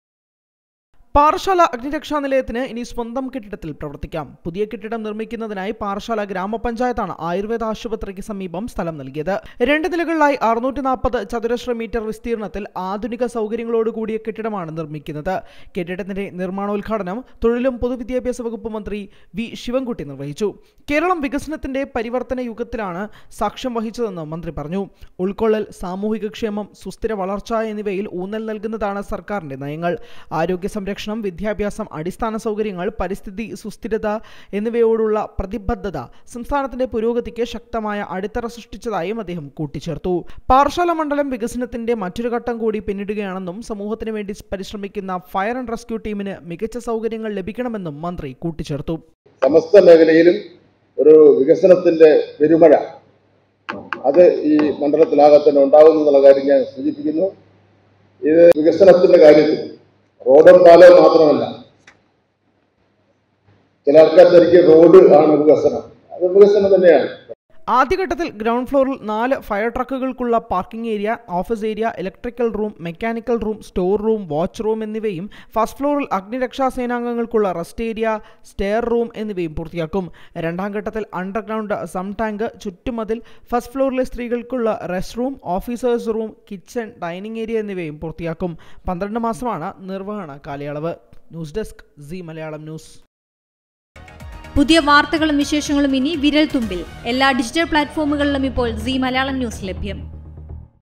Parassala | പാറശാല അഗ്നിരക്ഷാനിലയത്തിന് ഇനി സ്വന്തം കെട്ടിടം
Parassala | പാറശാല അഗ്നിരക്ഷാനിലയത്തിന്റെ പുതിയ മന്ദിരത്തിന്റെ നിർമാണോദ്ഘാടനം വി ശിവൻകുട്ടി നിർവഹിച്ചു